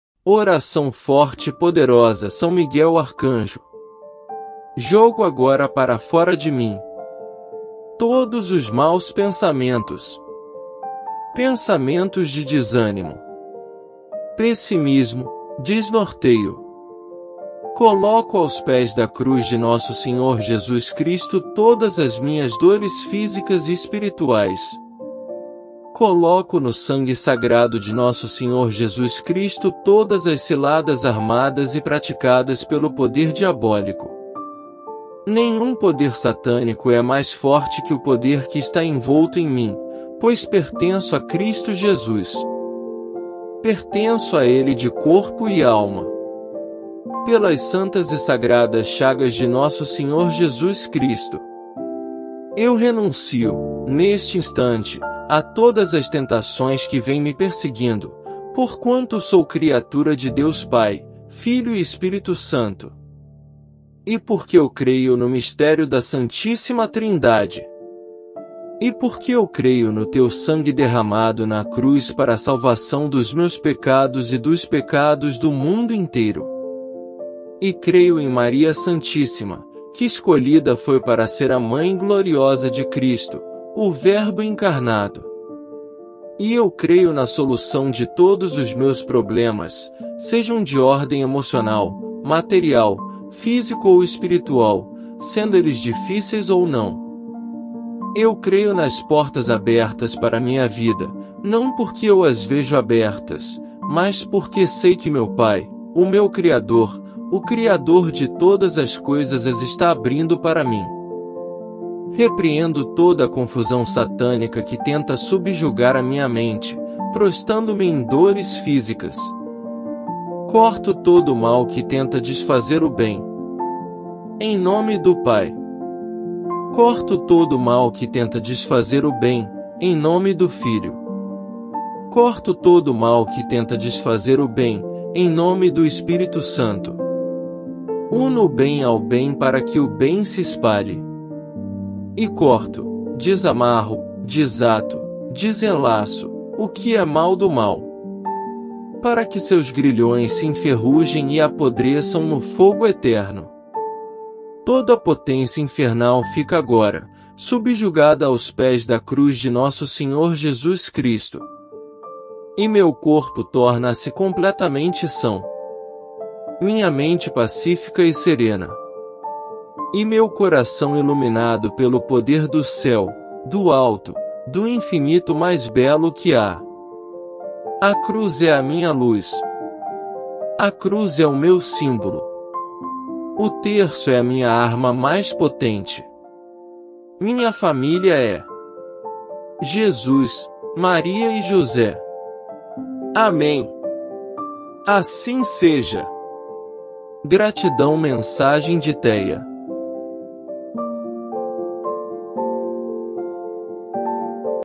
Oração Forte e Poderosa.São Arcanjo Miguel E Mestre Jesus, interceda por nós /Espiritualidade.
Oração forte e poderosa, Arcanjo Miguel, Mestre jesus/Espiritualidade. Direcione-nos e nos liberte, Companheiro de todos que desejam  se libertar e evoluir. Todos que clamam por ele são atendidos .
Gratidão!